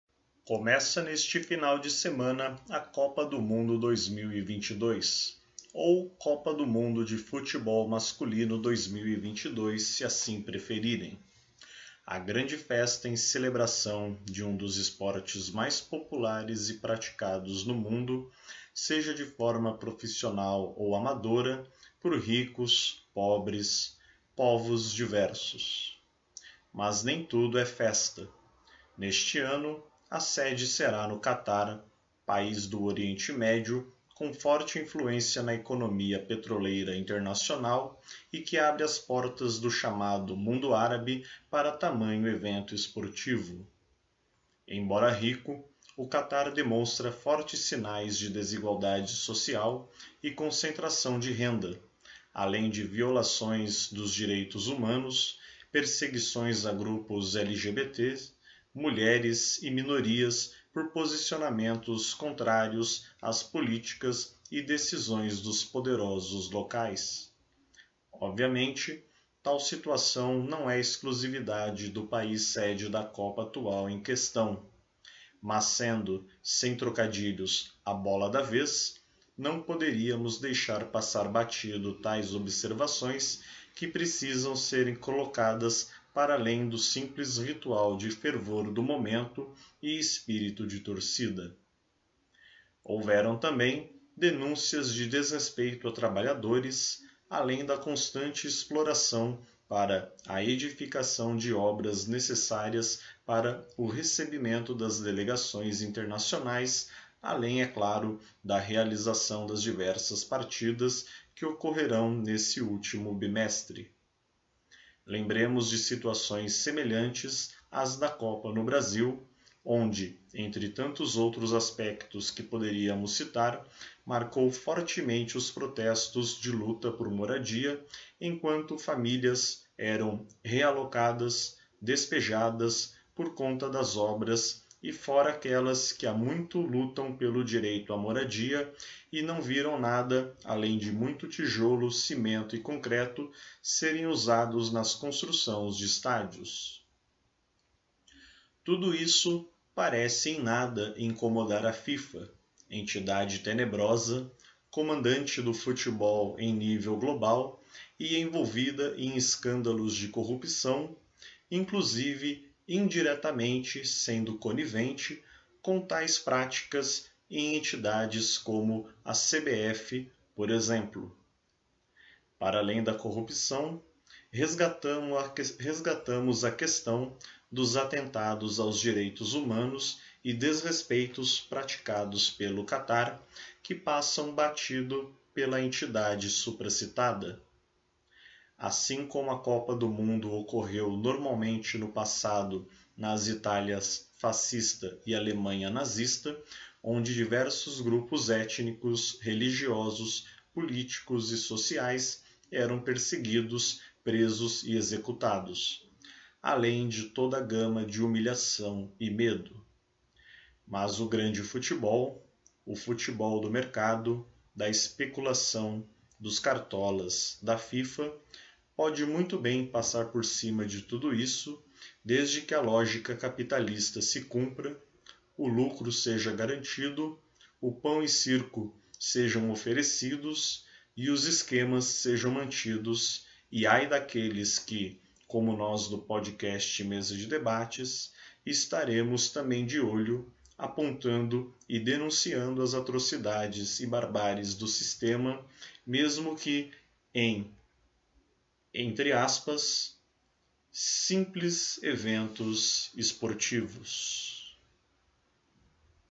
Mesa de debates apresenta: Copa do Mundo no Catar de 2022
André Bontempo comenta a Copa do Mundo no Catar de 2022 e toda problemática envolvendo questão política, econômica , social e de direitos humanos.